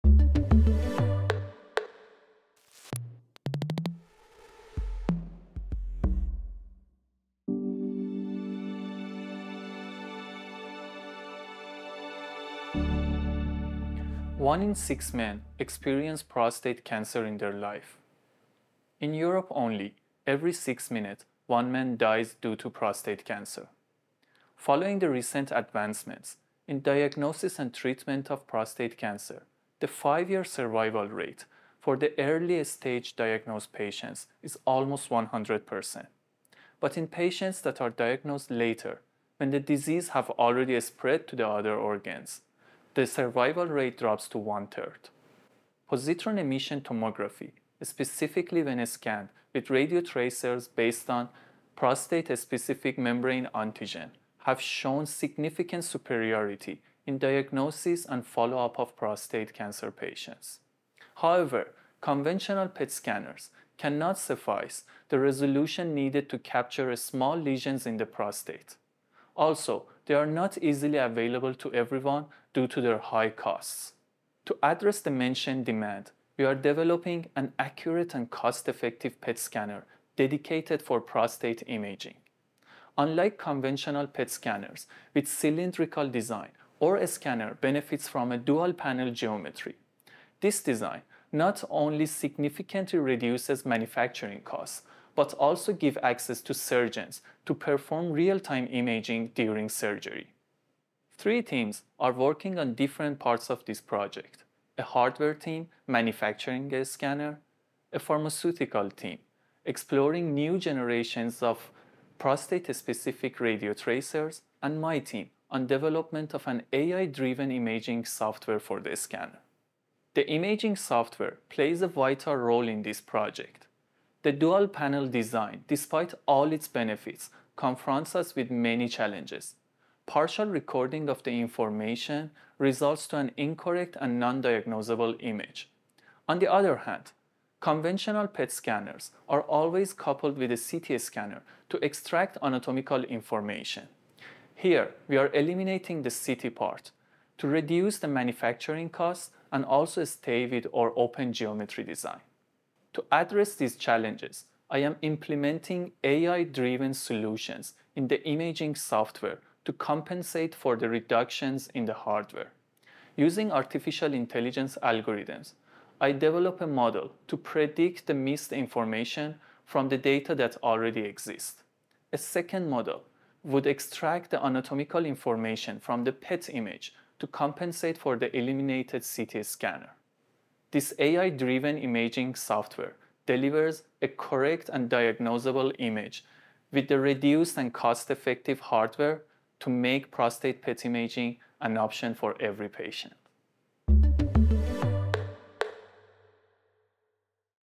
PhD Booster 2024: Prostate inspection: defeat cancer at inception
Mehdi Amini is one of the 2024 winners of the "PhD Booster" programme launched by the Faculty of Medicine, which aims to promote young talent in biomedical research.

Mehdi Amini is a PhD student at the University of Geneva (UNIGE), collaborating closely with the Hôpitaux Universitaires Genève.

Originally from Iran, Mehdi holds a background in Physics and a Master’s degree in Medical Physics. His research primarily explores the intersection of artificial intelligence (AI) and radiology, with a particular focus on molecular imaging. Currently, his PhD thesis is centered on developing a PET scanner specifically designed for prostate imaging. A significant component of his work involves leveraging AI-driven software to overcome the limitations posed by cost-effective, reduced hardware. The goal of his research is to pioneer a new generation of PET scanners that are both more affordable and precisely tailored to the unique requirements of prostate imaging, potentially making this advanced technology accessible to all patients with prostate cancer.